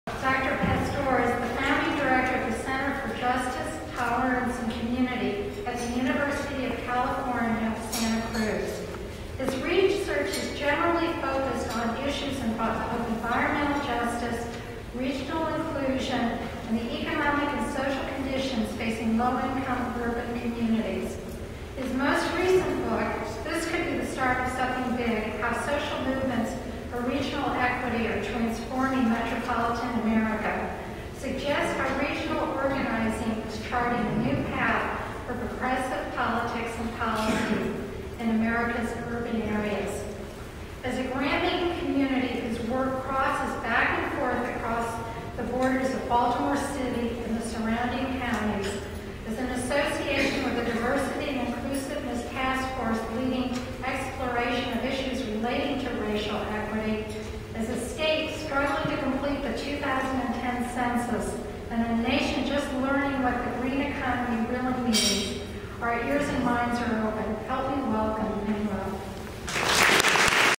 Betsy Nelson Introduces Manuel Pastor (ABAGAM 1 of 10)
Association of Baltimore Area Grantmakers President Betsy Nelson introduces Dr. Manuel Pastor at the ABAG annual meeting, May 12, 2010.